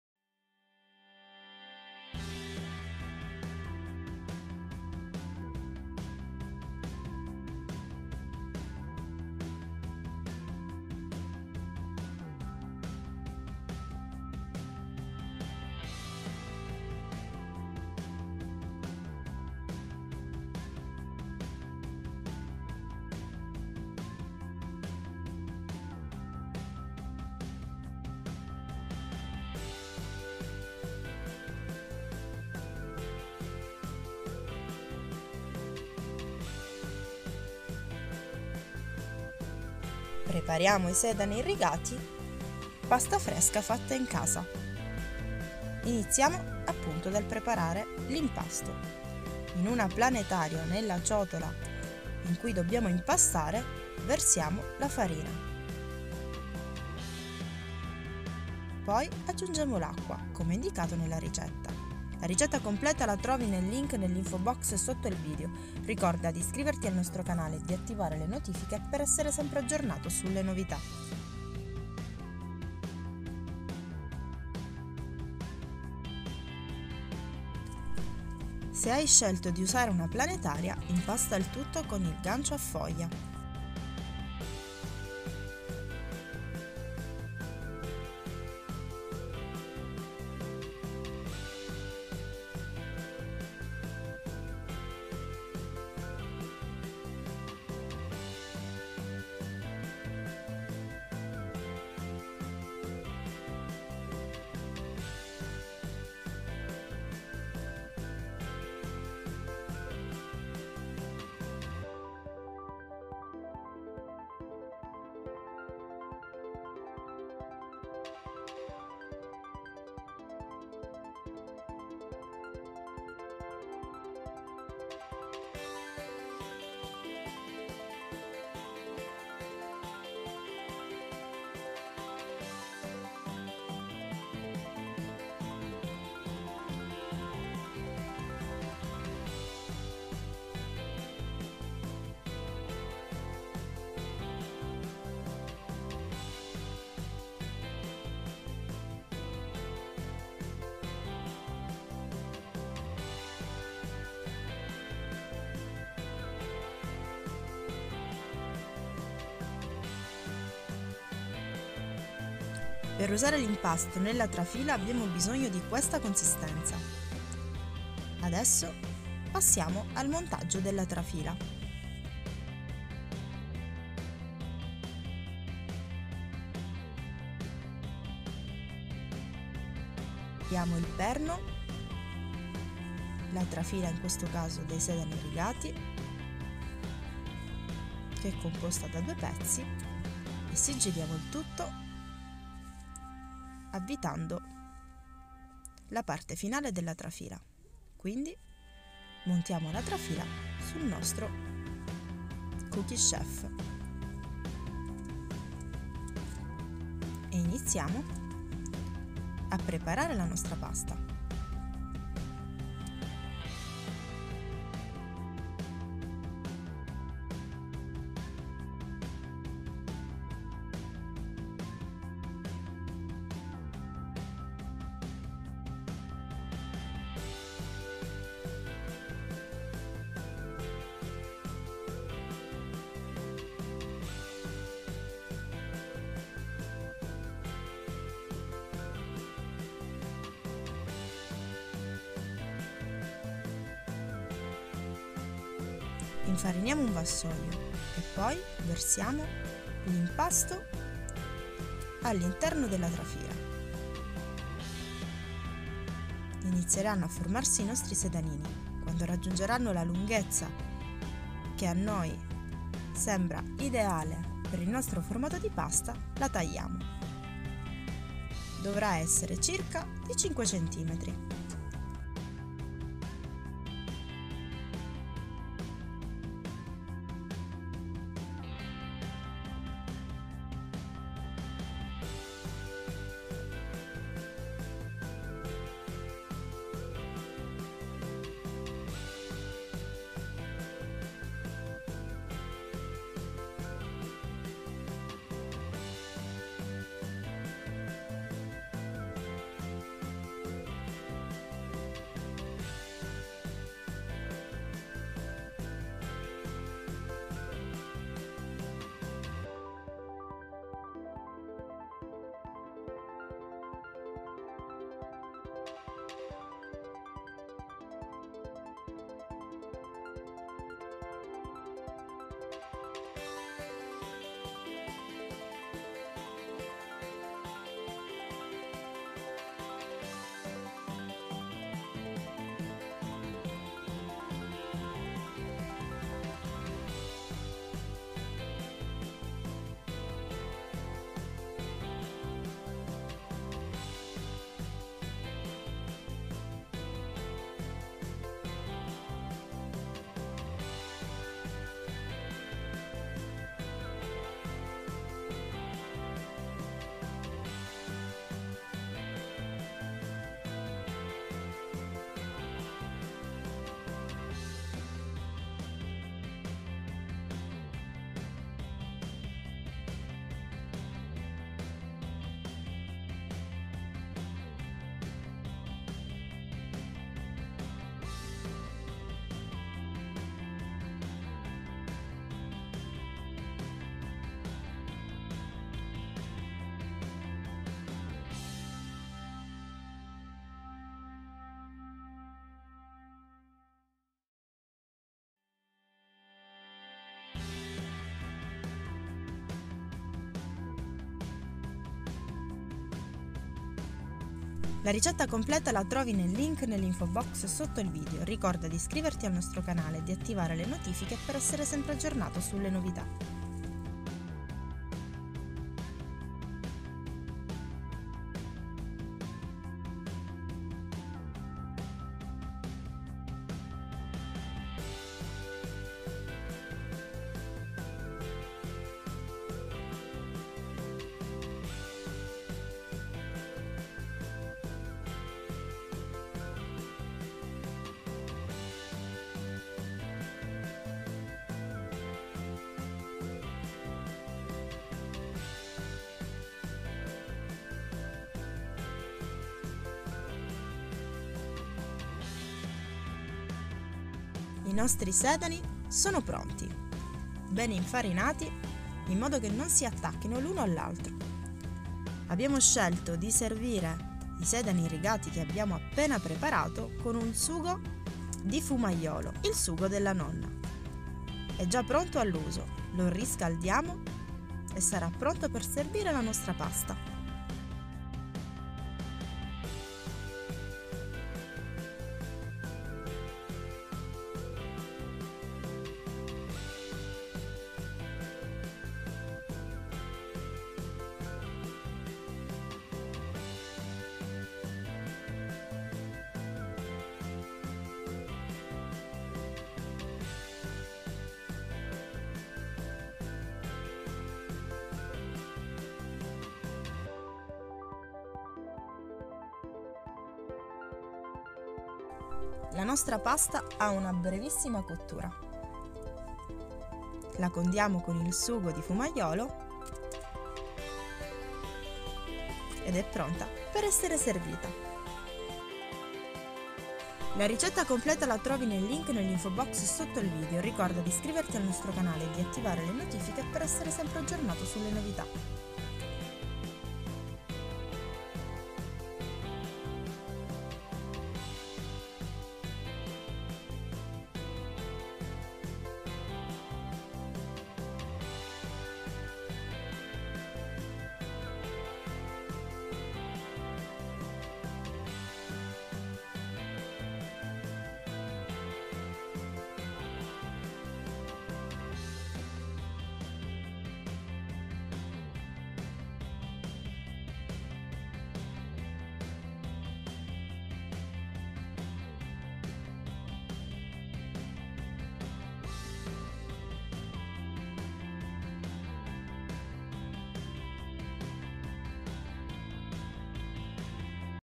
Sedanini rigati pasta fatta in casa Kenwood cooking chef torchio per la pasta trafila al bronzo
Credo che vi ho già detto in qualche ricetta di pasta fresca che a casa mia la faccio tutte le settimane. Ora mai di domenica e di obbligo a casa mia. La preparo in diversi formati da ravioli, tagliatelle o anche diverse forme.
Oggi vi faro conoscere questa ricetta di Sedanini rigati pasta fatta in casa da preparare nella planetaria kenwood cooking chef con torchio per la pasta e trafila di bronzo.
Visto che abbiamo iniziato la collaborazione con bellissima e buonissima azienda Fumaiolo che ci ha offerto i loro sughetti Fumaiolo già pronti ho pensato che la miglio ricetta per gustarli sarà proprio con Sedanini rigati pasta fatta in casa. Vi posso dire che il risultato mi ha stupita il sughetto e veramente molto buono sembra proprio appena fatto fresco. Consigliatissimo.
Per chi ha voglio di provarlo clicca qui per acquistarlo nel negozio.

➡️ Vuoi ricevere aggiornamenti con Telegram?

➡️ Vuoi altri video?